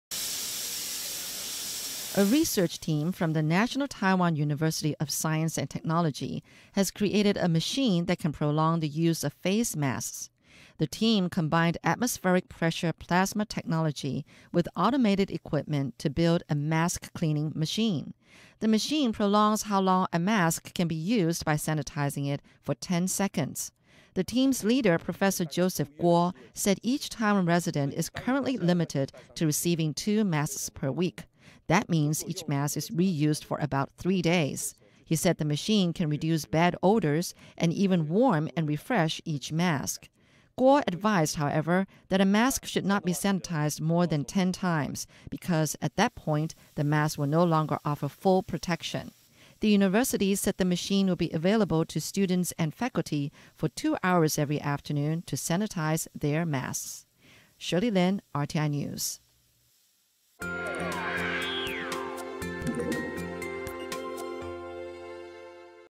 Taiwanese university invents machine to prolong face mask life | Taiwan News | RTI
A research team from the National Taiwan University of Science and Technology has created a machine that can prolong the use of face masks. The team combined atmospheric pressure plasma technology with automated equipment to build a mask-cleaning machine. The machine prolongs how long a mask can be used by sanitizing it for 10 seconds.